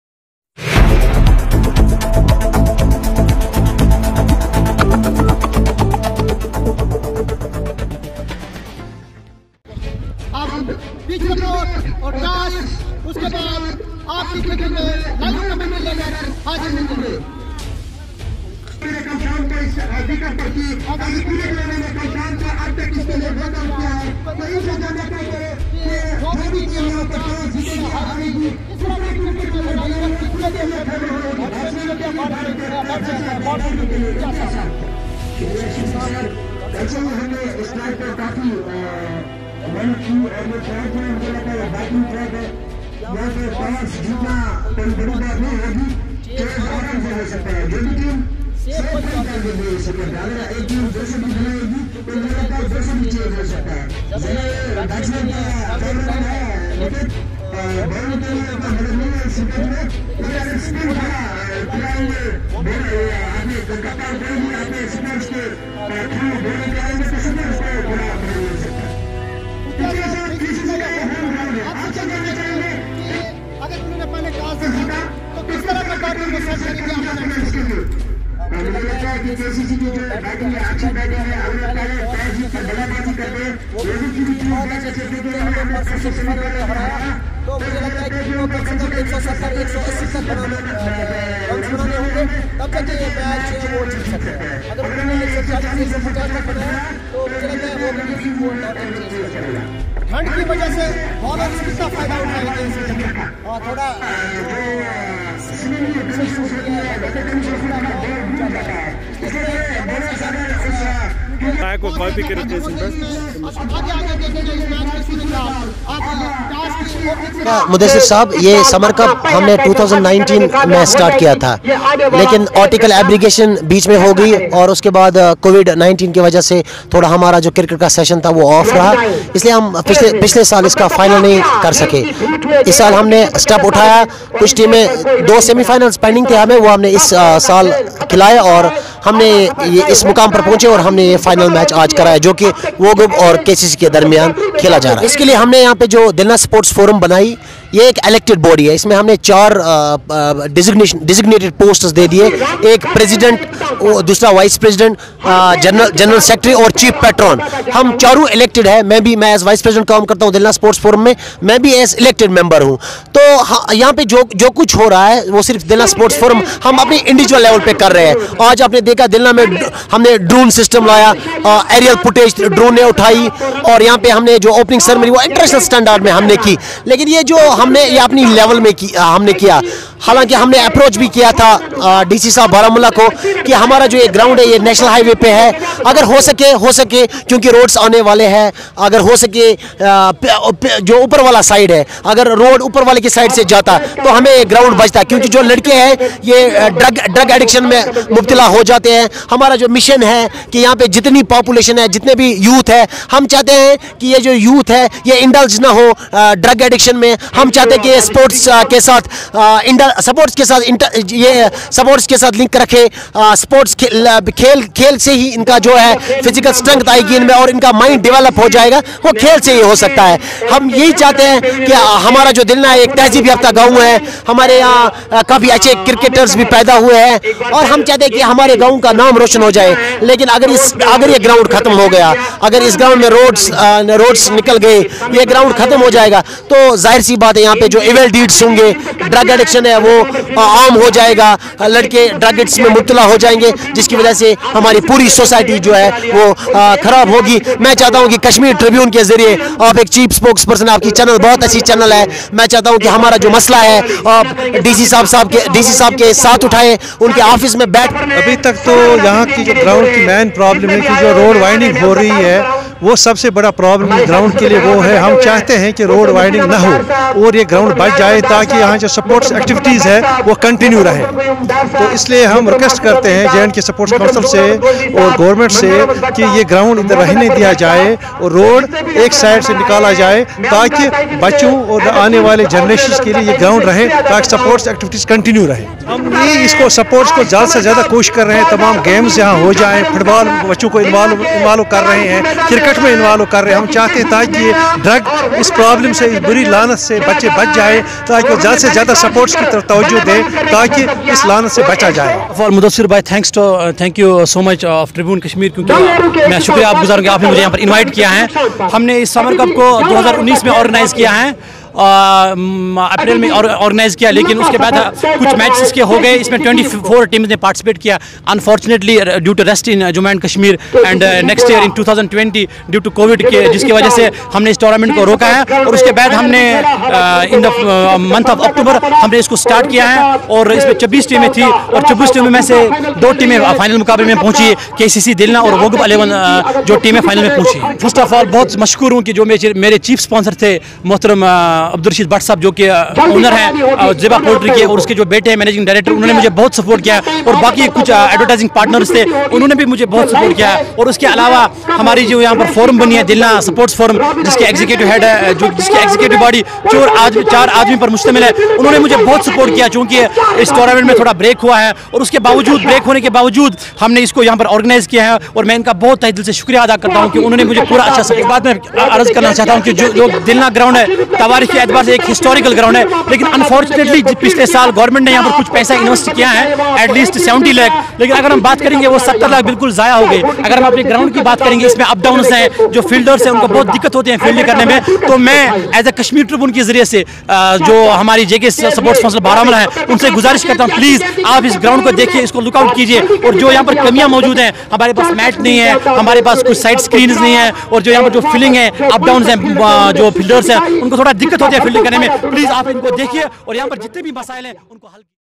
Summer cup 2019- 20 final match was played between kcc Delina and wagub sports.
summer cup 29-20 was played between kcc Delina and wagub sports.